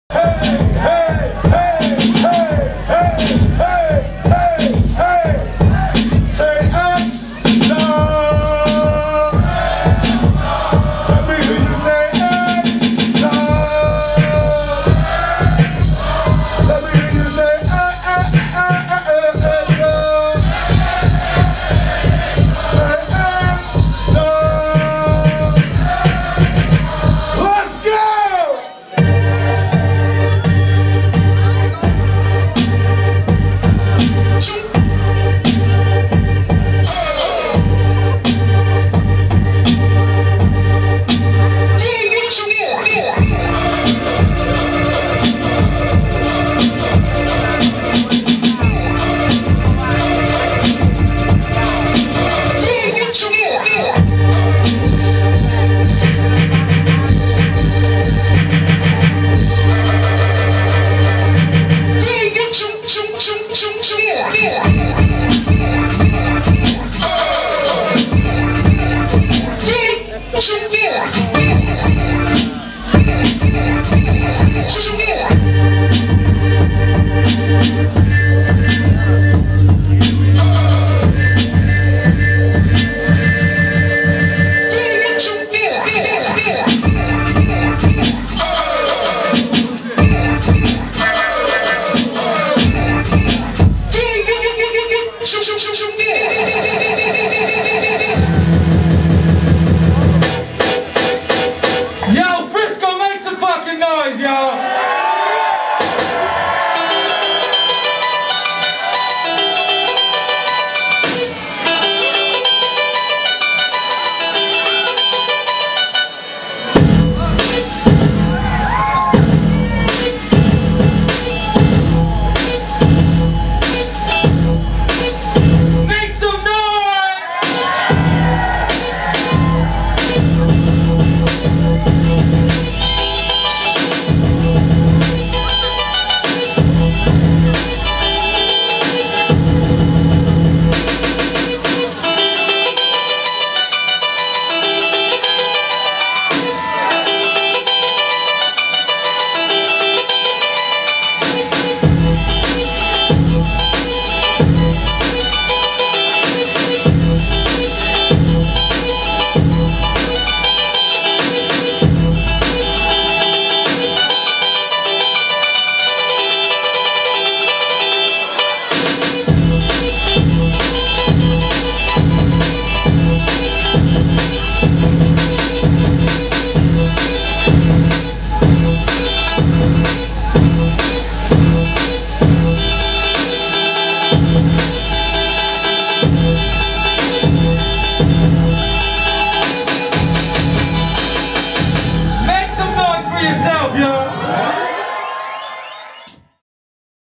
Video: Exile Live @ Left Fest In San Francisco (1/28/2010)
Last Thursday, Exile took the stage before Crown Royal and The Souls of Mischief to bless the crowd with another dope MPC/DJ performance. He got on stage to rap a few songs too and Ill have those from both of his San Francisco shows later this week.